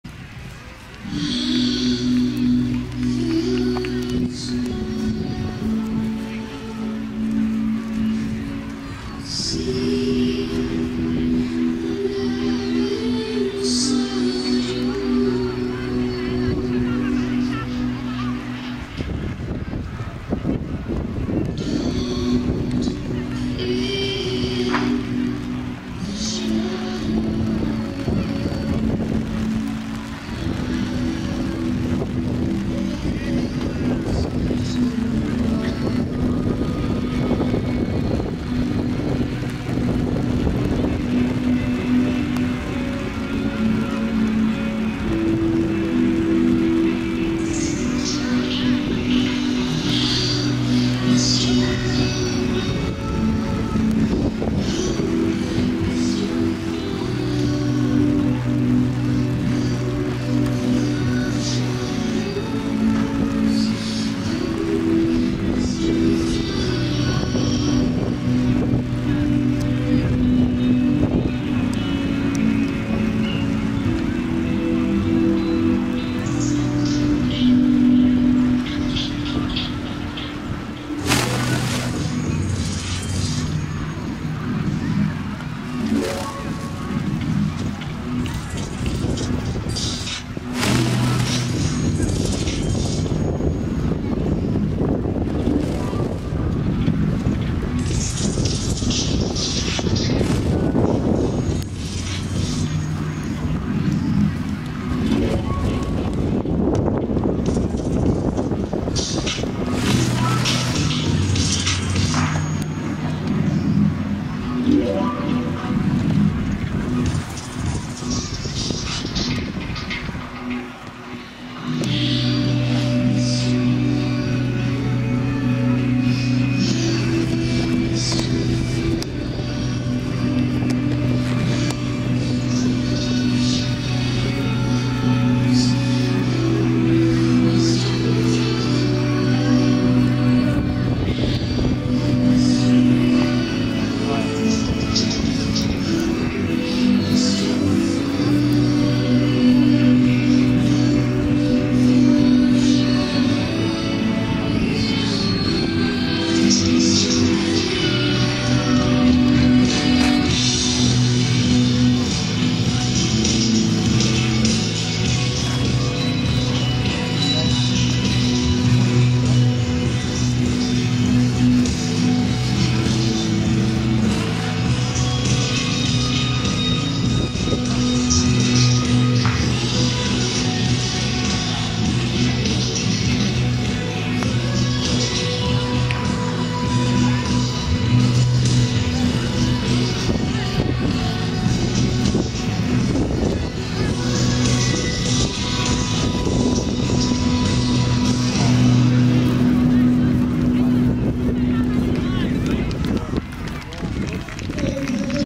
Tristan Underwood - Windscape Kite Festival - Rev Routine #2
Tristan Underwood performing a Rev routine at the 2013 Windscape Kite Festival in Swift Current, Saskatchewan.